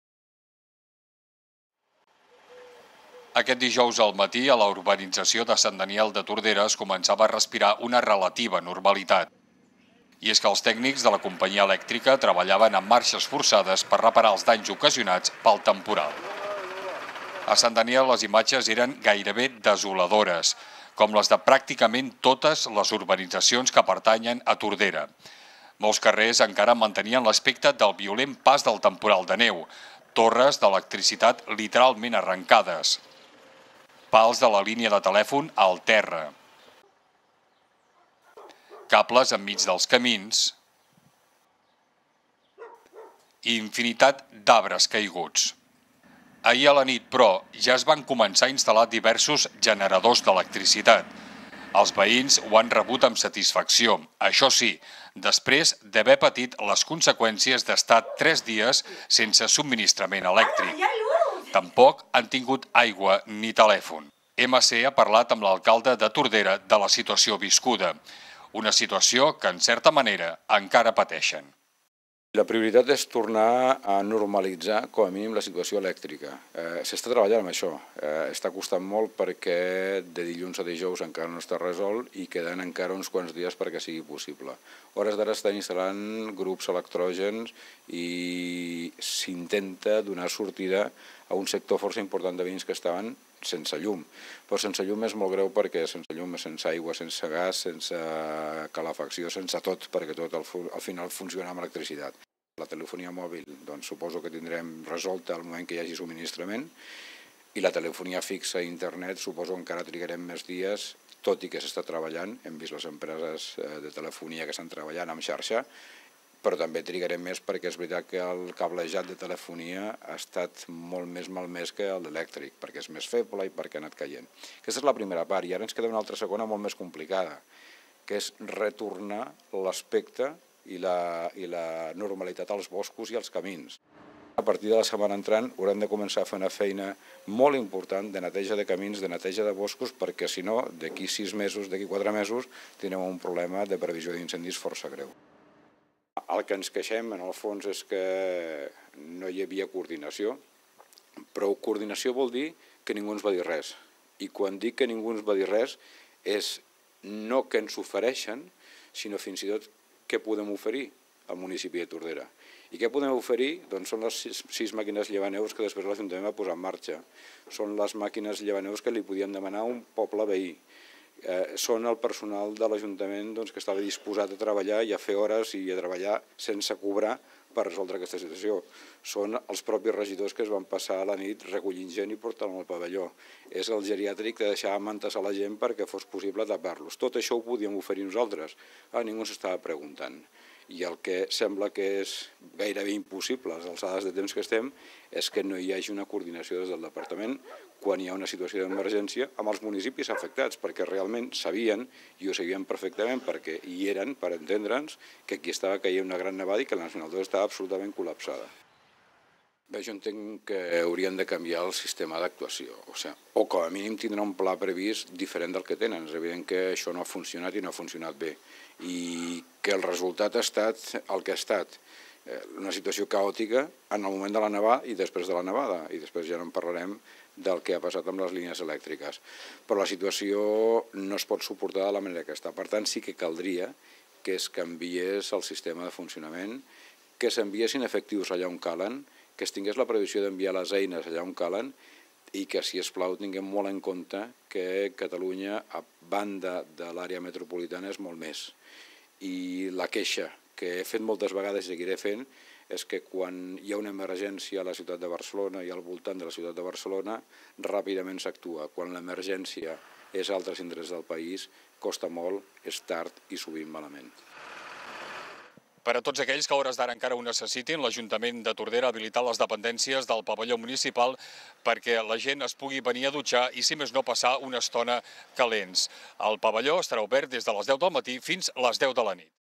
va tornant la llum
Aquest dijous al matí, a l'urbanització Sant Daniel de Tordera es començava a respirar una relativa normalitat. I és que el tècnics de la companyia elèctrica treballaven a marxes forçades per reparar els danys ocasionats pel temporal.
A Sant Daniel les imatges eren gairebé desoladores, com les de pràcticament totes les urbanitzacions que pertanyen a Tordera. Molts carrers encara mantenien l'aspecte del violent pas del temporal de neu: torres d'electricitat literalment arrancades, pals de la línia de telèfon al terra, cables enmig dels camins i infinitat d'arbres caiguts.
Ahir a la nit, però, ja es van començar a instal·lar diversos generadors d'electricitat. Els veïns ho han rebut amb satisfacció. Això sí, després d'haver patit les conseqüències d'estar tres sense subministrament elèctric. Tampoc han tingut aigua ni telèfon.
MC ha parlat amb l'alcalde de Tordera sobre la situació viscuda aquests dies. Una situació que, en certa manera, encara pateixen. Joan Carles García (CiU), ha remarcat que ara per ara la prioritat és restablir el subministrament elèctric a les cases afectades. L'alcalde reconeix que, tot i els treballs, encara es trigarà uns dies a recuperar la telefonia mòbil i el gas ja que aquests són serveis que també depenen de l'electricitat. El que sí ha volgut remarcar amb insistència és la necessitat urgent de retornar l'aspecte i la normalitat als boscos i els camins. De cara a la setmana que ve s'hauran de començar a netejar ràpidament perquè sinó a l'estiu hi haurà un problema greu de prevenció d'incendis.
Pel que fa al dia de la nevada, l'alcalde ha criticat la manca d'informació als ajuntaments. Ningú ens va dir res, ha assegurat García. L'alcalde ha volgut deixar clar que en cap moment se'ls va consultar ja no només sobre quines eren les necessitats sinó què podien oferir des del consistori. Joan Carles García ha recordat que es disposaven de sis màquines llevaneus i que es van haver de posar en marxa per iniciativa pròpia de l'Ajuntament. Però García no només ha fet referència a la maquinària sinó que també ha manifestat que tot el personal de l'Ajuntament estava disposat a treballar i a fer les hores que fes falta sense cobrar per a resoldre aquesta situació. En aquest sentit ha recordat que durant el temporal van ser els mateixos regidors els qui van rescatar les persones aïllades a la carretera. L'alcalde ha criticat la manca de coordinació entre el departament d'Interior i els diferents cossos de seguretat i emergències i els municipis afectats. Sabien perfectament que estava caient una gran nevada i la N-II estava col·lapsada, ha dit. Haurien de canviar el sistema d'actuació o canviar l'existent ja que ha quedat molt clar que no ha funcionat, ha sentenciat Joan Carles García.
L'alcalde s'ha queixat de l'evident centralisme que s'aplica en segons quin tipus de situacions: sisplau, tinguem molt en compte que Catalunya és molt més que a l'àrea metropolitana. Quan hi ha una emergència al voltant de la ciutat de Barcelona, s'actua ràpidament. Quan l'emergència és en d'altres indrets del país, costa molt, és tard i sovint es fa malament.

Per als veïns de les urbanitzacions de Tordera que encara ho necessitin, l'Ajuntament facilita un dels vestuaris del pavelló esportiu municipal. D'aquesta manera, les persones afectades podran dutxar-se i estar una estona escalfats. L'horari és de deu del matí a deu de la nit.